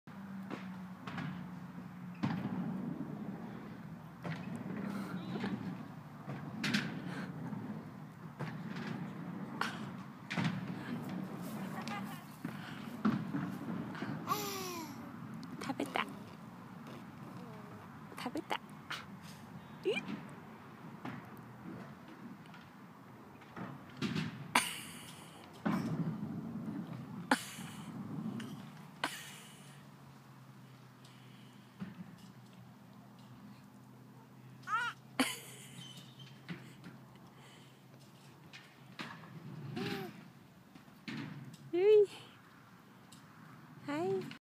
8 months old Lui eating grass ルイが草食べた
公園にピクニックに行ったら、今まで怖がっていた芝生を気にせずハイハイして、一人で遊んでました。
しばらくして様子を見たら、草食べてゲーコしそうになってた。
可愛いなあ♡